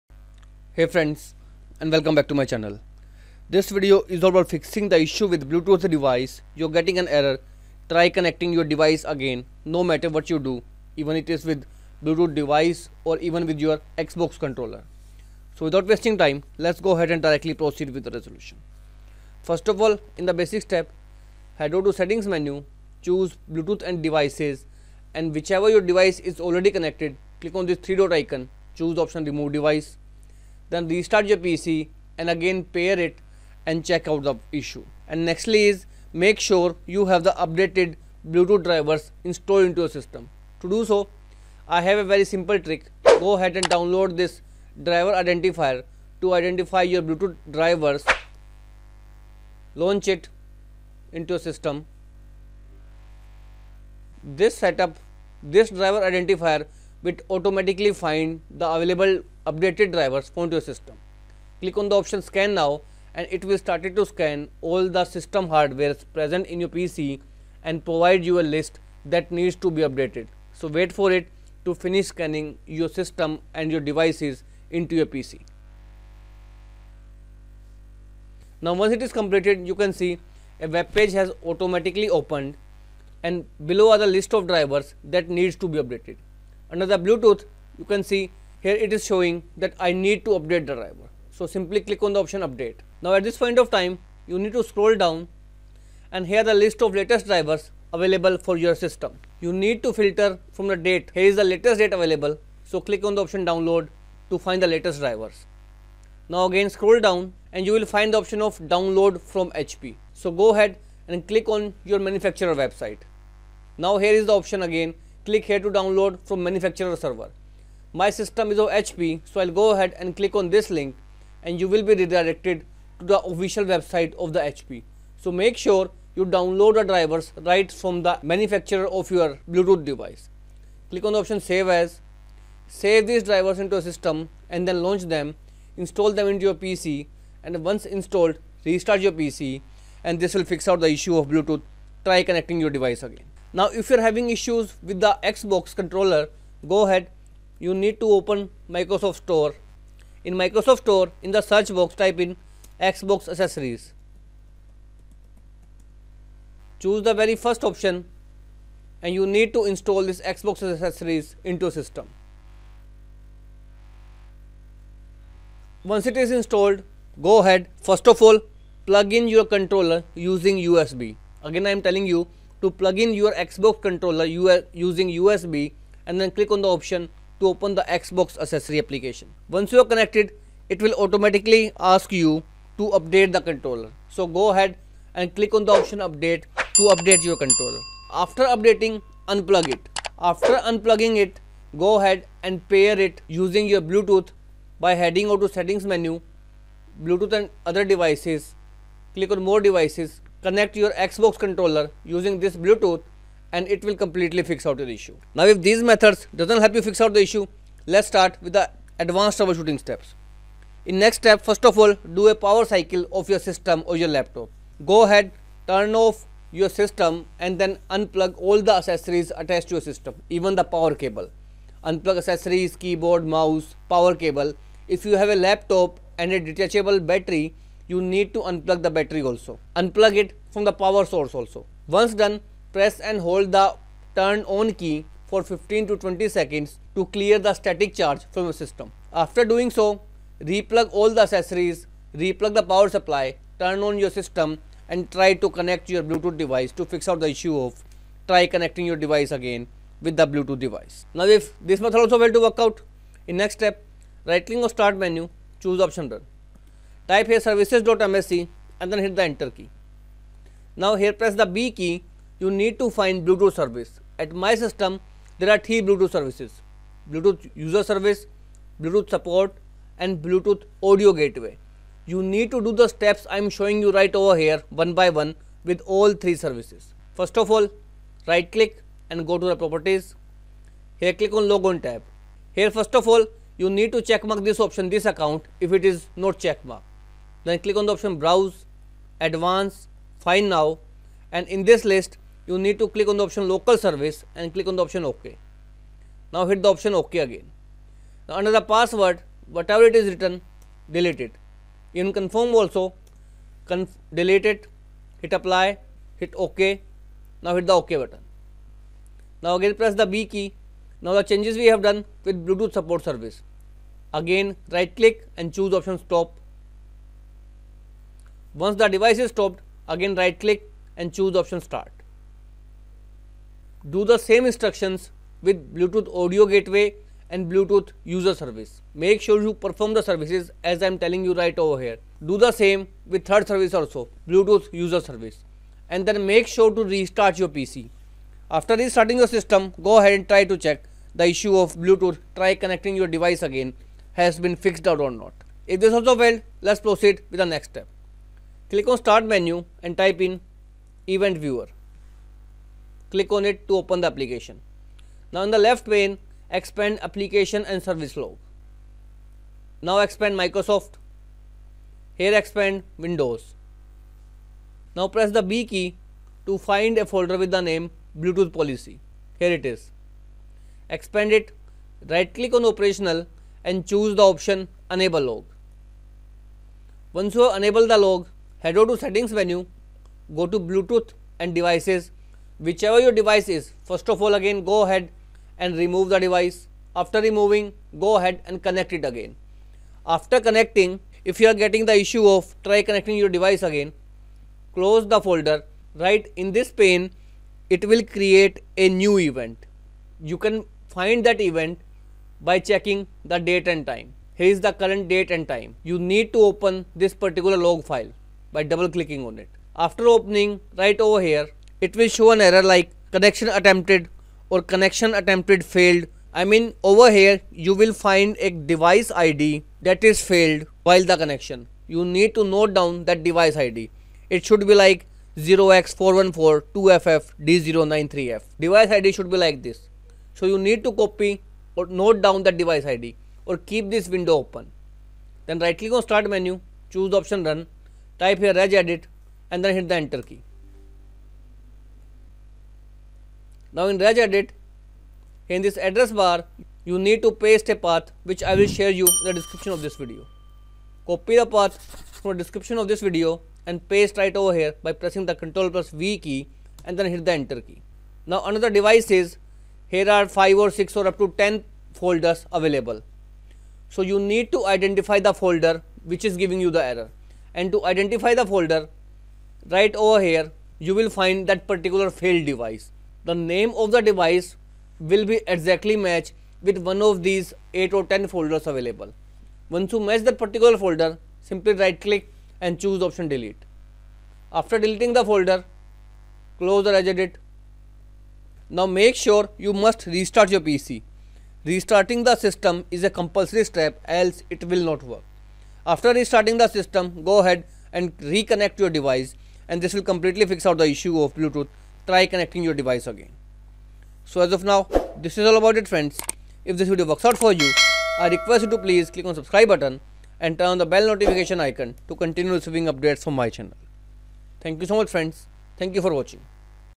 Bluetooth try connecting your device again Fix
This video is all about fixing the issue of bluetooth try connecting your device again while connecting Xbox controller, apple airpods or any other bluetooth device in your windows pc.
So watch the complete tutorial as i have included the advanced steps to fix try connecting your device again.

00:00 Introduction
00:18 Method 1 by doing basic steps of repairing and updating drivers
02:23 Method 2 by doing power cycle of pc
03:28 Method 3 fixing the xbox controller issue
04:10 Method 4 by using the services menu
05:45 Method 5 by using the event viewer

Address to paste in regedit:
Computer\HKEY_LOCAL_MACHINE\SYSTEM\CurrentControlSet\Services\BTHPORT\Parameters\Devices